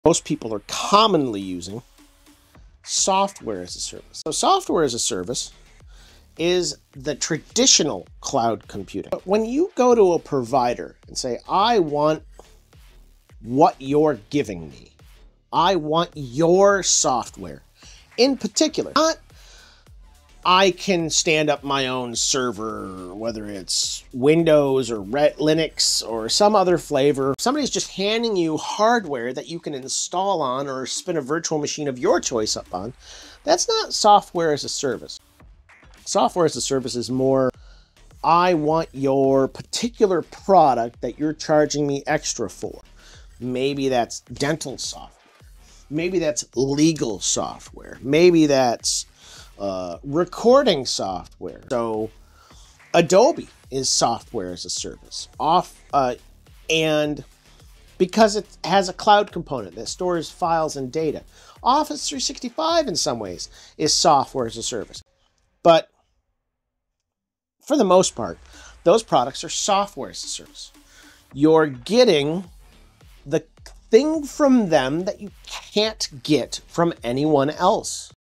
SaaS Explained: Understanding Software as a Service
Software as a Service (SaaS) is when a provider delivers their software directly to the user. This differs from simply providing hardware or a virtual machine. SaaS means using a specific product, like Adobe or Office 365, offered by a vendor.